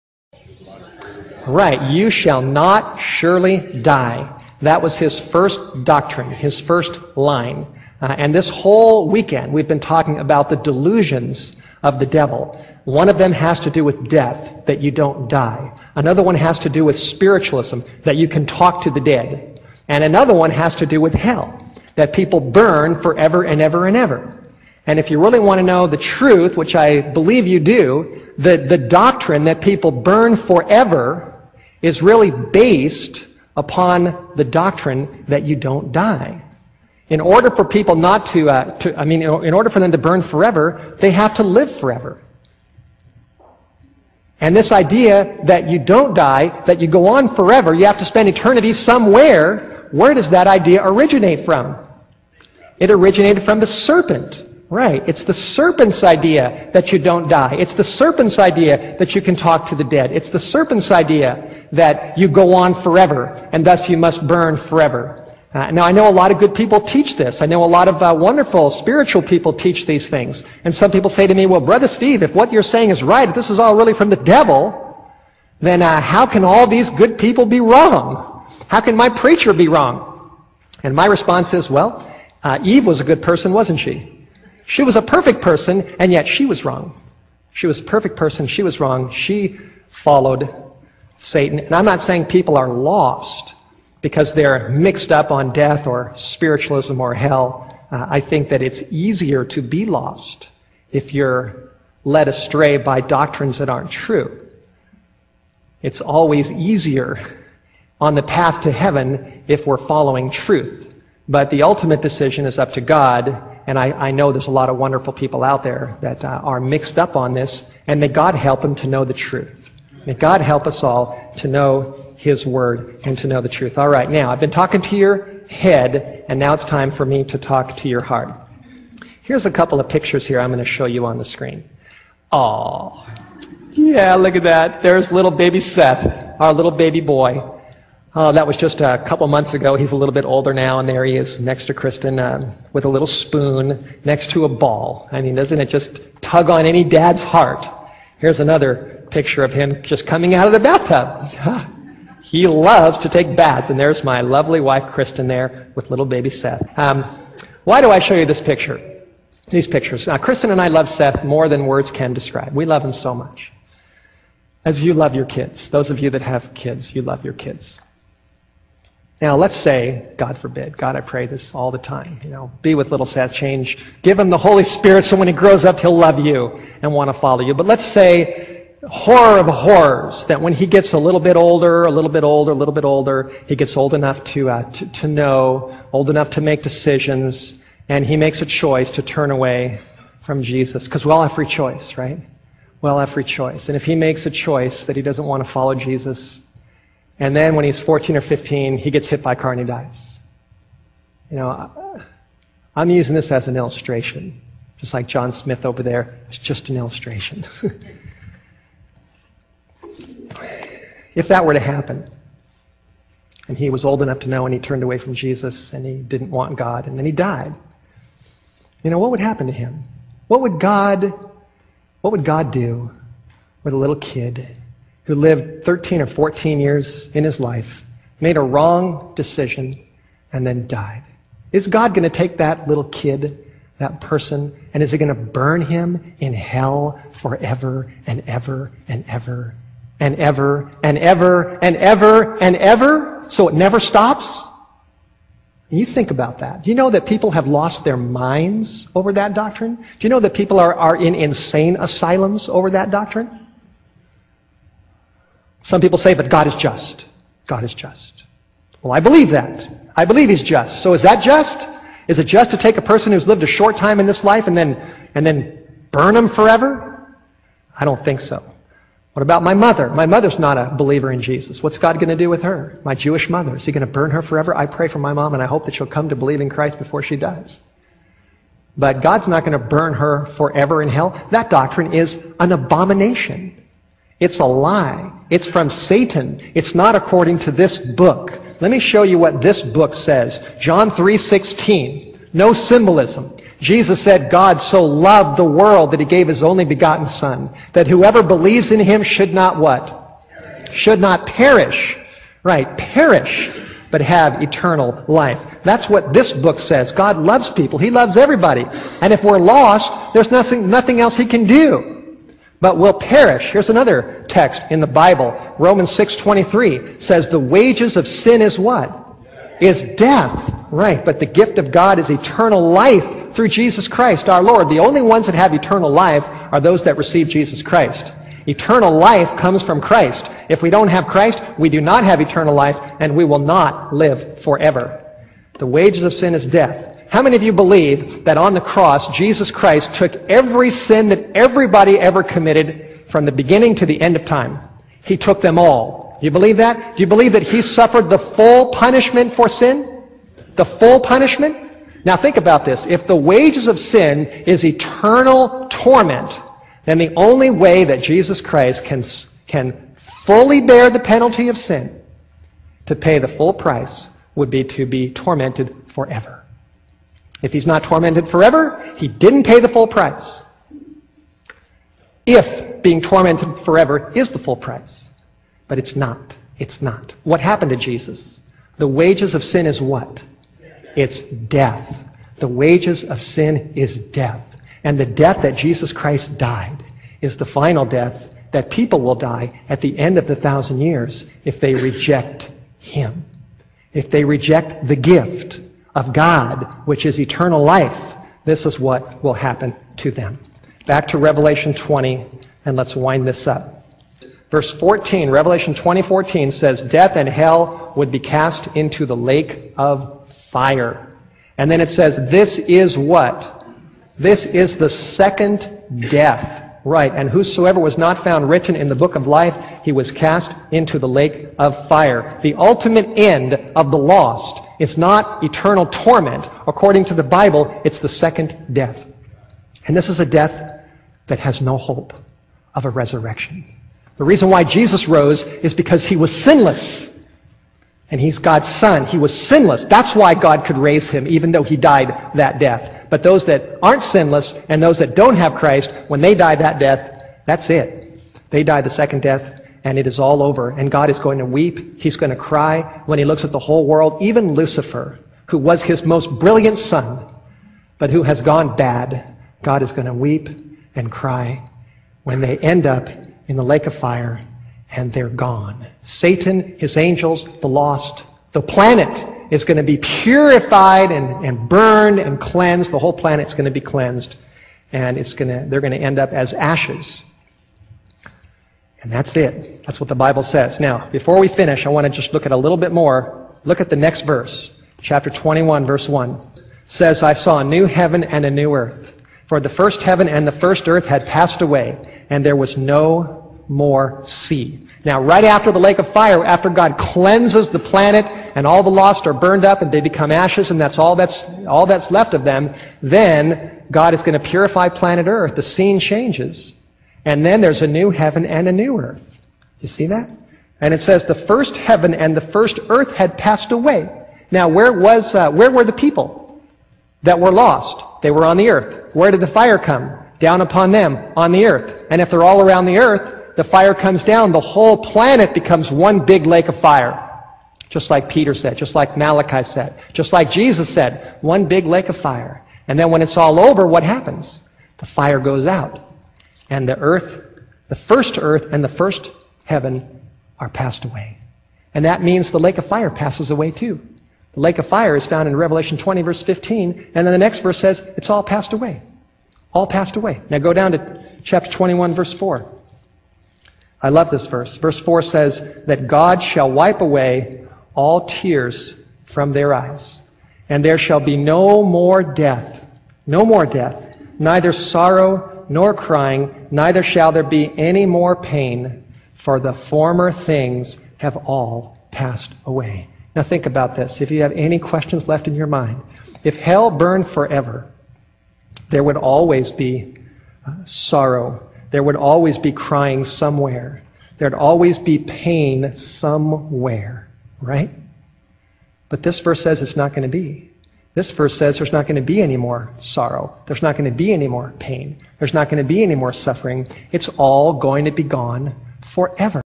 The Hot Topic of Hell Part 6 of 6 [MIRROR]
What does the Bible teach about hell and the fate of the wicked? What did Jesus Christ say about it? Find out in this Bible study.